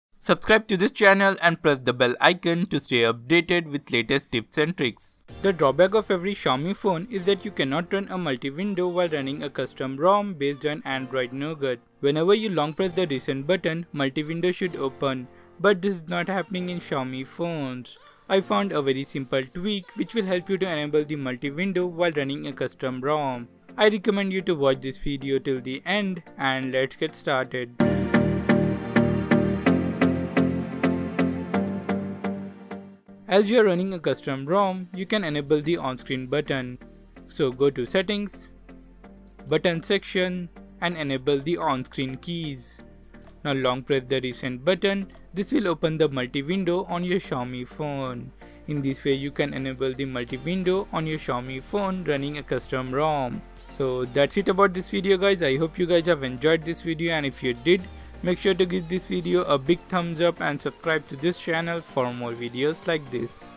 Enable Multi Window in Xiaomi Phones(Redmi Note 4, Note 3, 3S, Mi4,Mi5,etc)
The drawback of every Xiaomi phone is that they do not support multi-window while running a custom ROM based on Android  N 7.0 Nougat or Android O 8.0 Oreo. I found a very simple tweak to enable Multi Window in Xiaomi Phones(Redmi Note 4, Note 3, 3S, Mi4,Mi5,etc)

My Gear:
New Camera
Old Camera